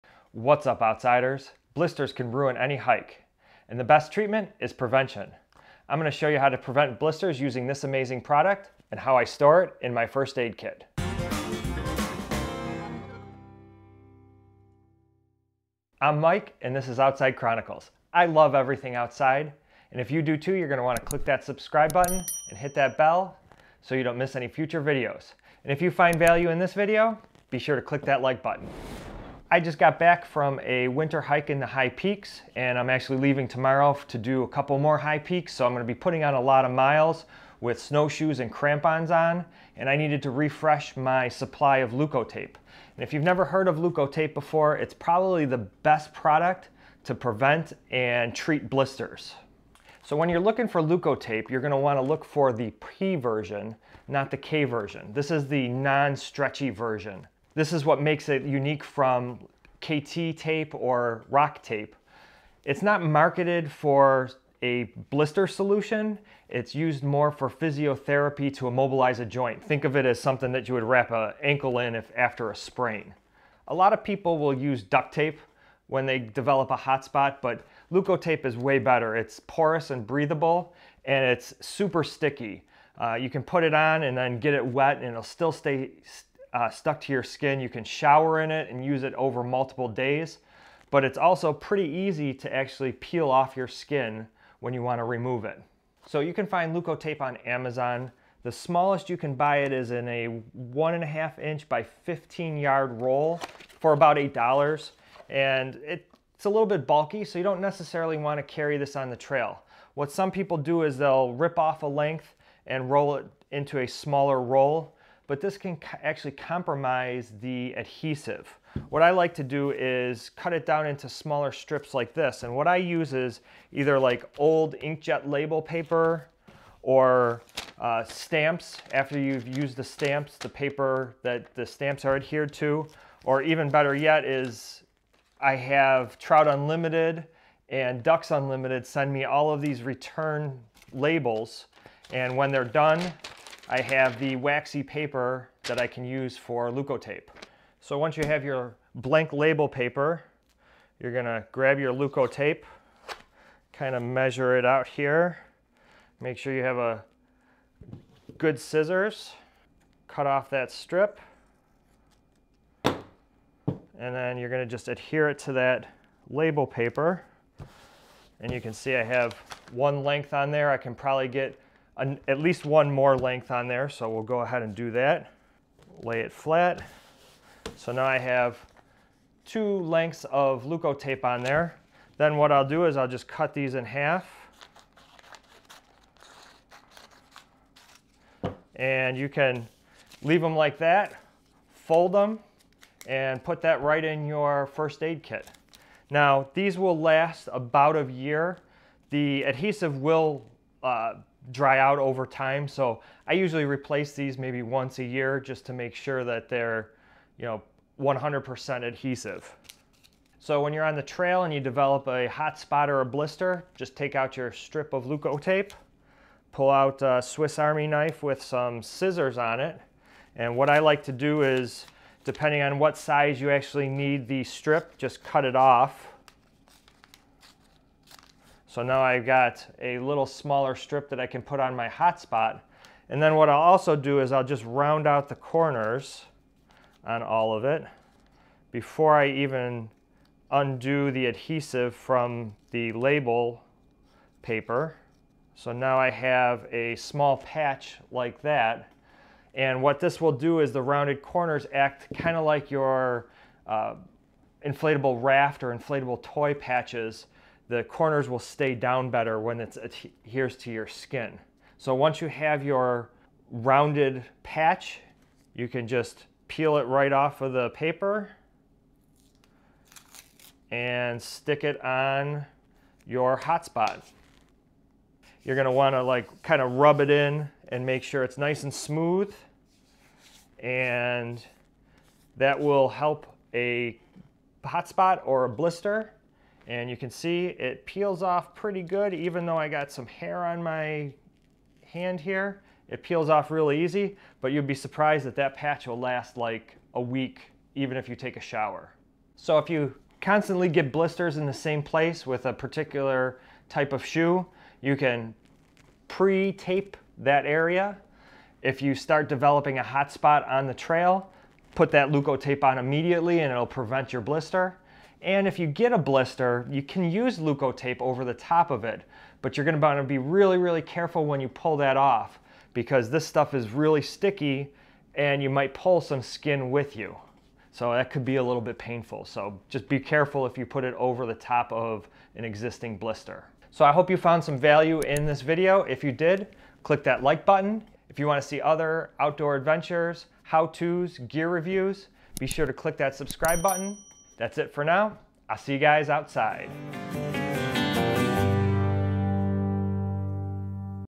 AVOID BLISTERS WHEN HIKING // Leukotape can prevent and treat hotspots and blisters on the trail
Leukotape can prevent and treat blisters on the trail.  Leukotape can be used to avoid blisters by applying it to hotspots while on the trail.  You can prevent blisters by pre-taping any areas that are prone to hotspots prior to a long dayhike or during a multi-day hike.  Leukotape can also treat blisters that have already formed.